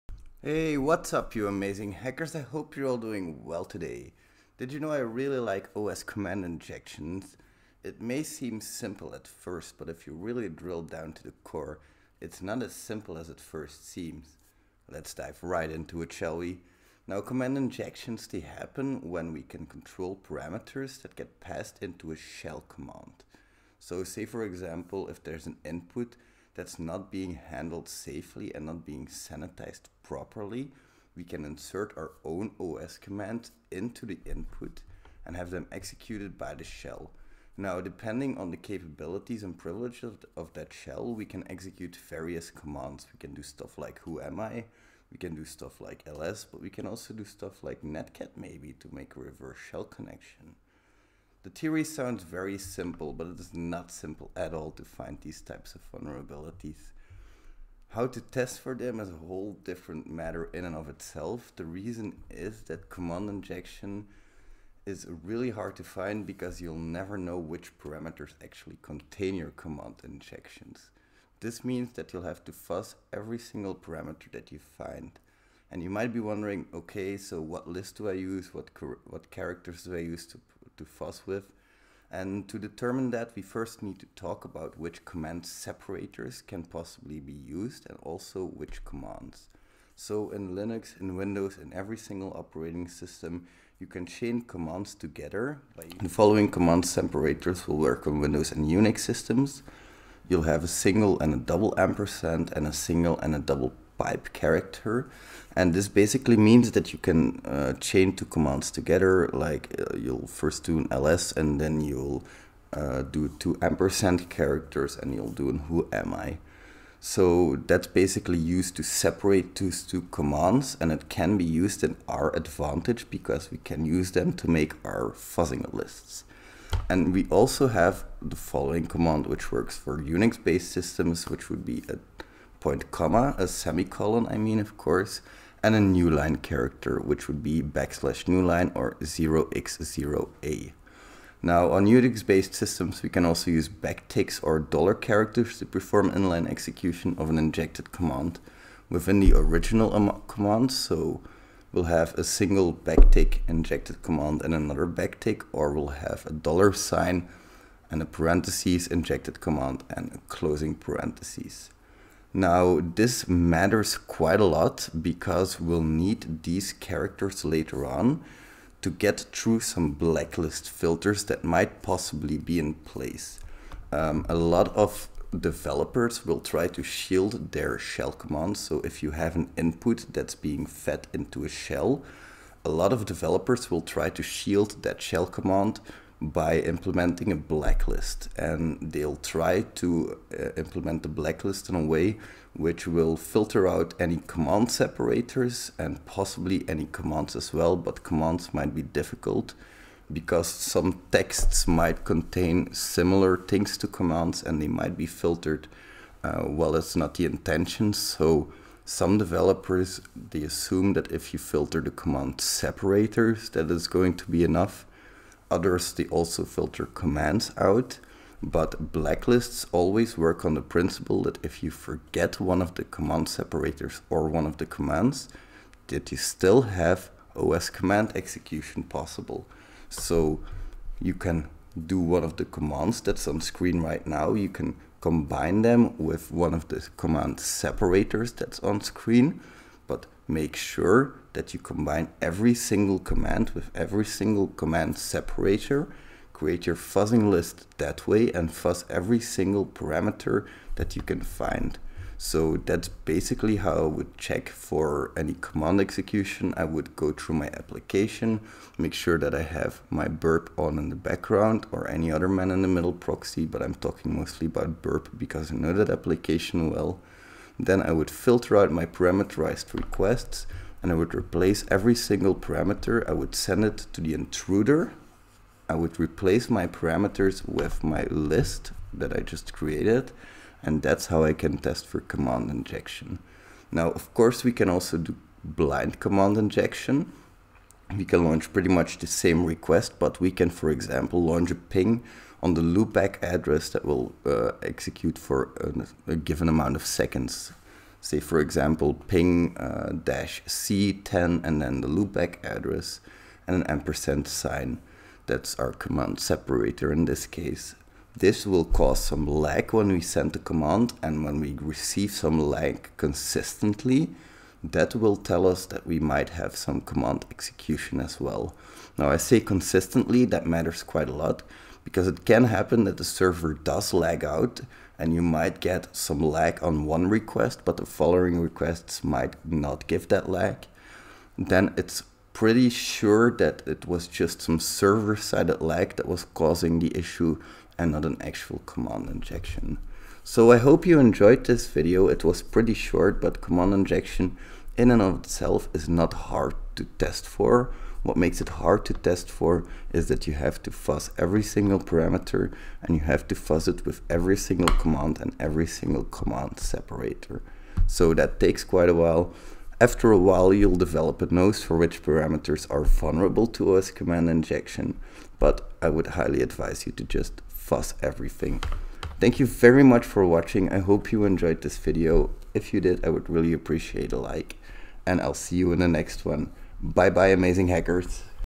Uncle Rats Ultimate Guide To Finding OS Command Injection
OS Command injections may seem like a simple vulnerability but they are anything but easy to find. Uncle Rat explains how he looks for them in this short value packed guide.

thexssrat